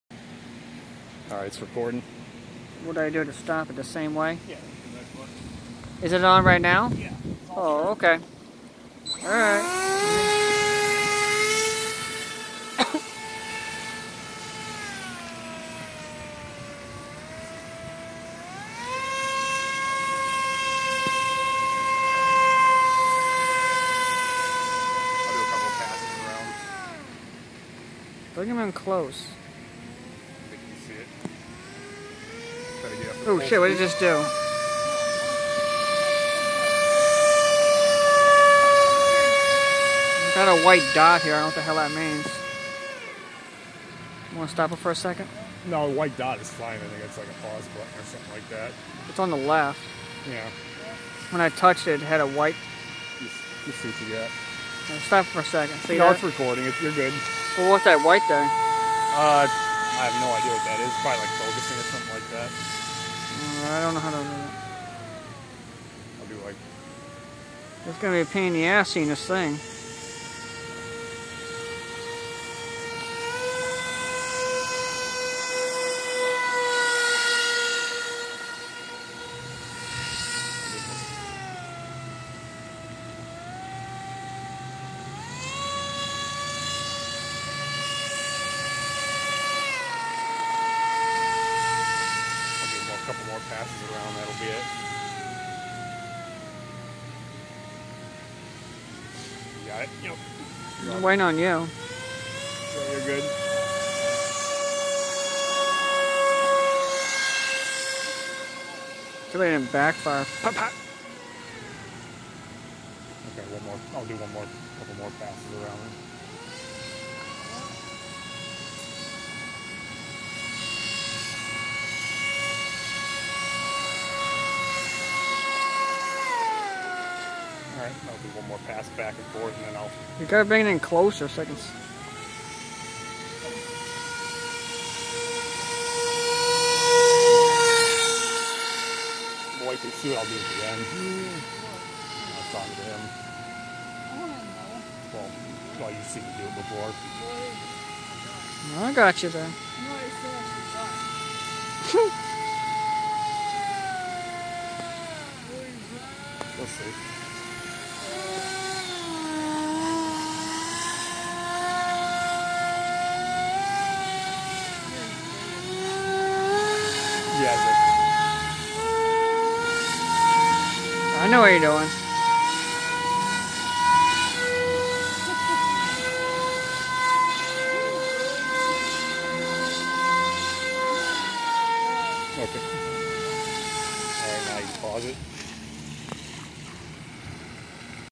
Aquacraft Cajun Commander run
A run of the awesome Cajun Commander air boat.